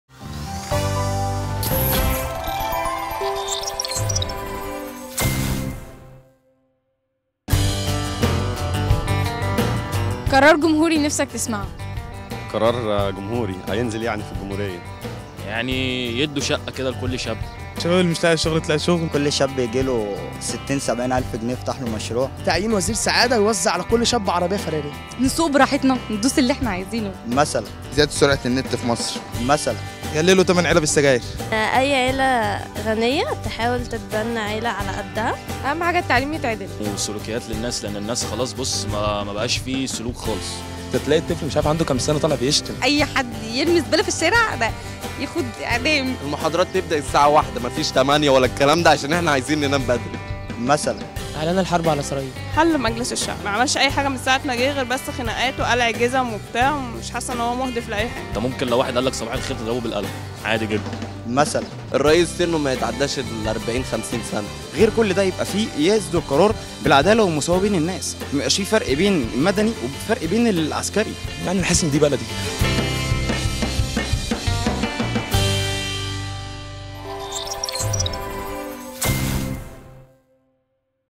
دوت مصرl إيه القرار الجمهوري اللي نفسك تسمعه ؟
وسط كل التحديات و الأخبار، سألنا الناس لو في قرار جمهوري تحب تسمعه، ممكن يبقى إيه ؟
إسمع إجاباتهم

تقرير: منى مجدي و نور المصري ومحمود ترك

مونتاج: مينا ميخائيل